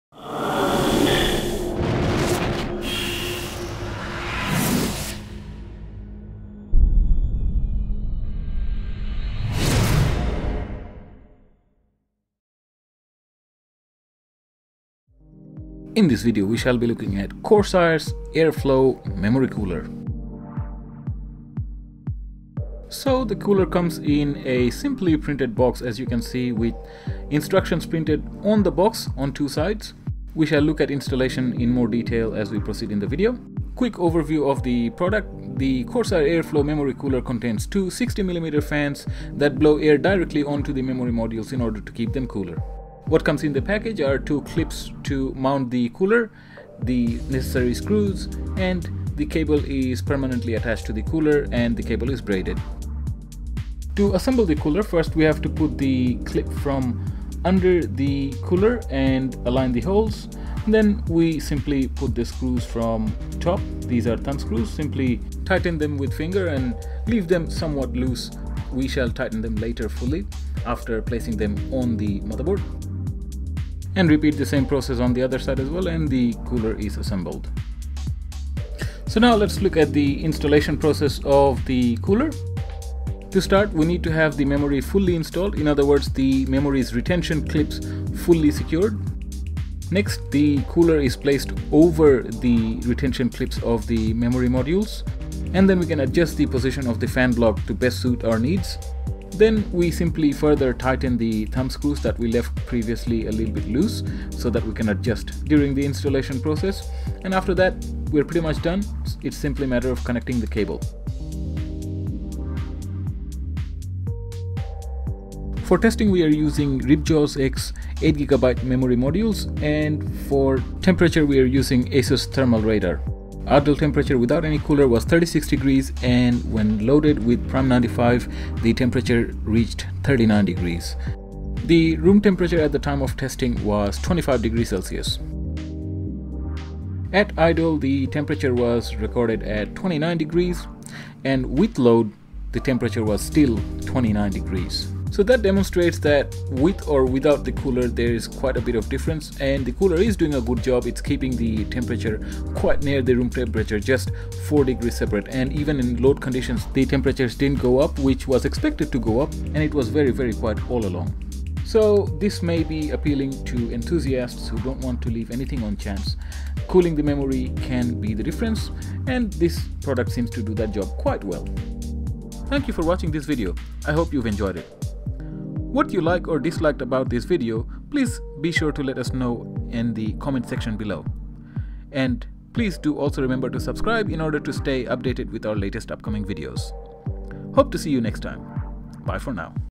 Corsair AIRFLOW II Ram Cooler Review
In pursuit of keeping everything cool, the memory often gets overlooked. With the Corsair Airflow cooler that need will easily be met.
2x60mm fans do a good job of keeping the memory modules very close to the room temperature.